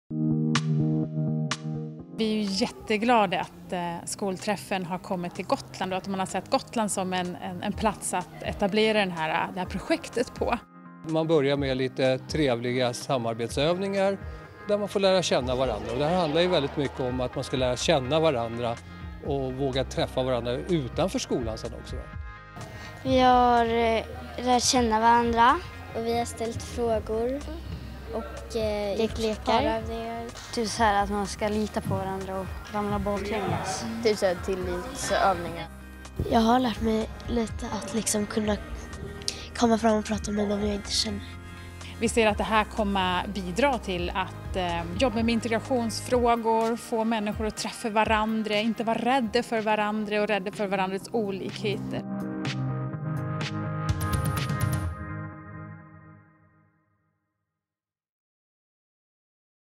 Skolträffen Gotland Trailer
Skolträffen är ett projekt där skolor från olika områden får mötas för att bearbeta fördomar genom samtal, föreläsningar, samarbetsövningar och besök på varandras skolor.